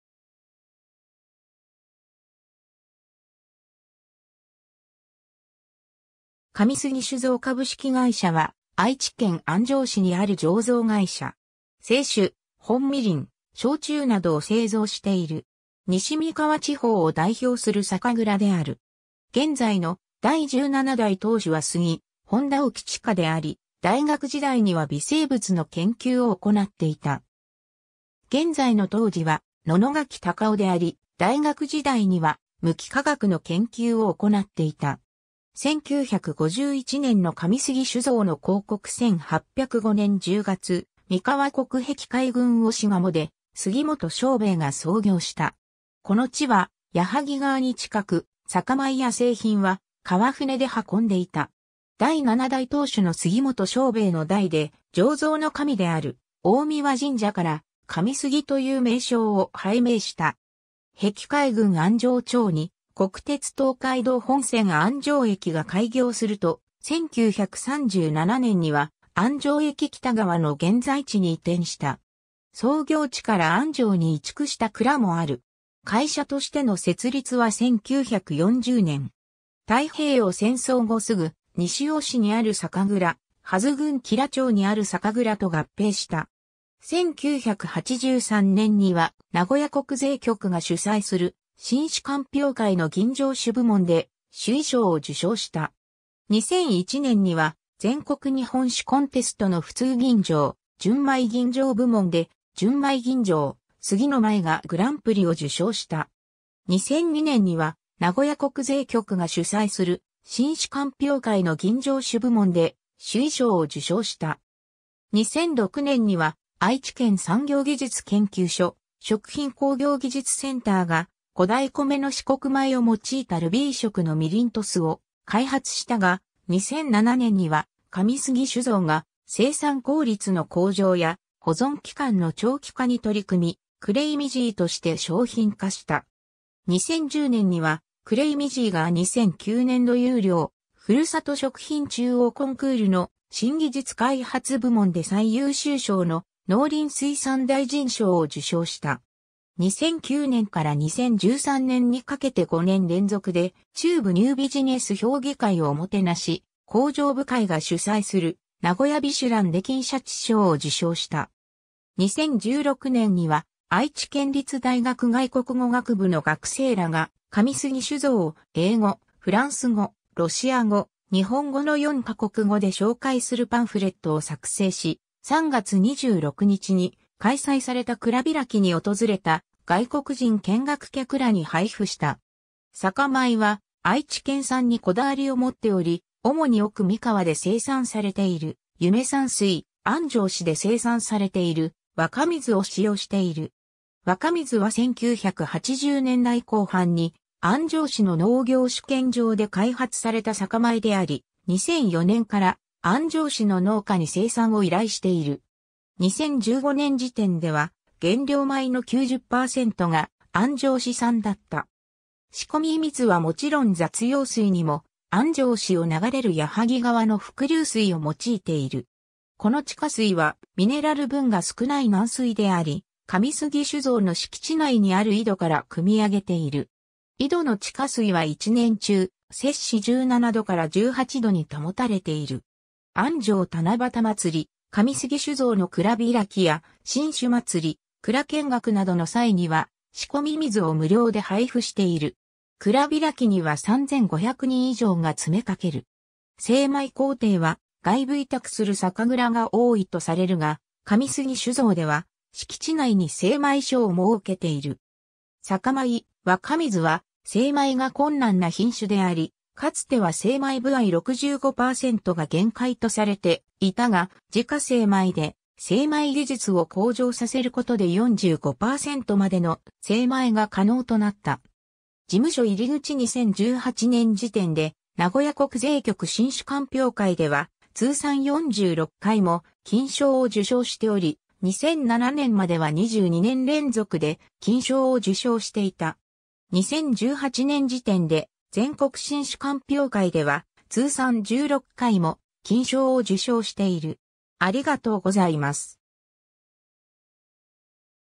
神杉酒造
老舗企業_(江戸時代創業)
神杉酒造株式会社（かみすぎしゅぞう）は、愛知県安城市にある醸造会社。
清酒、本みりん、焼酎などを製造している。
西三河地方を代表する酒蔵である。
現在の第17代当主は杉本多起哉であり、大学時代には微生物の研究を行っていた。
現在の杜氏は野々垣高雄であり、大学時代には無機化学の研究を行っていた。
 1951年の神杉酒造の広告 1805年（文化2年）10月、三河国碧海郡鴛鴨（後の碧海郡寿恵野村、続いて碧海郡上郷町、現在の豊田市鴛鴨町）で杉本庄兵衛（名跡であり、2代目以降も庄兵衛を名乗った）が創業した。
この地は矢作川に近く、酒米や製品は川船で運んでいた。
第7代当主の杉本庄兵衛の代で、醸造の神である大神神社（大和国桜井）から神杉という名称を拝名した。
碧海郡安城町に国鉄東海道本線安城駅が開業すると、1937年（昭和12年）には安城駅北側の現在地に移転した。
創業地から安城に移築した蔵もある。
会社としての設立は1940年（昭和15年）。
太平洋戦争後すぐ、西尾市にある酒蔵、幡豆郡吉良町にある酒蔵と合併した。
 1983年（昭和58年）には名古屋国税局が主催する新酒鑑評会の吟醸酒部門で首位賞を受賞した。
2001年（平成13年）には全国日本酒コンテストの普通吟醸・純米吟醸部門で「純米吟醸・杉の舞」がグランプリを受賞した。
2002年（平成14年）には名古屋国税局が主催する新酒鑑評会の吟醸酒部門で首位賞を受賞した。
2006年（平成18年）には、愛知県産業技術研究所食品工業技術センターが古代米の紫黒米を用いたルビー色の味醂と酢を開発したが、2007年（平成19年）には神杉酒造が生産効率の向上や保存期間の長期化に取り組み、「Cremisi」として商品化した。
2010年（平成22年）には「Cremisi」が2009年度優良ふるさと食品中央コンクールの新技術開発部門で最優秀賞の農林水産大臣賞を受賞した。
 2009年（平成21年）から2013年（平成25年）にかけて5年連続で、中部ニュービジネス評議会おもてなし向上部会が主催する「なごや美酒欄」で金鯱賞を受賞した。
2016年（平成28年）には愛知県立大学外国語学部の学生らが、神杉酒造を英語・フランス語・ロシア語・日本語の4か国語で紹介するパンフレットを作成し、3月26日に開催された蔵開きに訪れた外国人見学客らに配布した。
 酒米は愛知県産にこだわりを持っており、主に奥三河で生産されている「夢山水」、安城市で生産されている「若水」を使用している。
「若水」は1980年代後半に安城市の農業試験場で開発された酒米であり、2004年（平成16年）から安城市の農家に生産を依頼している。
2015年（平成27年）時点では原料米の90%が安城市産だった。
 仕込み水はもちろん雑用水にも、安城市を流れる矢作川の伏流水（地下水）を用いている。
この地下水はミネラル分が少ない軟水であり、神杉酒造の敷地内にある井戸からくみ上げている。
井戸の地下水は一年中摂氏17度から18度に保たれている。
安城七夕まつり、神杉酒造の蔵開きや新酒まつり、蔵見学などの際には、仕込水を無料で配布している。
蔵開きには3500人以上が詰めかける。
 精米工程は外部委託する酒蔵が多いとされるが、神杉酒造では敷地内に精米所を設けている。
酒米「若水」は精米が困難な品種であり、かつては精米歩合65%が限界とされていたが、自家精米で精米技術を向上させることで45%までの精米が可能となった。
 事務所入口 2018年（平成30年）時点で、名古屋国税局新酒鑑評会では通算46回も金賞を受賞しており、2007年（平成19年）までは22年連続で金賞を受賞していた。
2018年時点で、全国新酒鑑評会では通算16回も金賞を受賞している。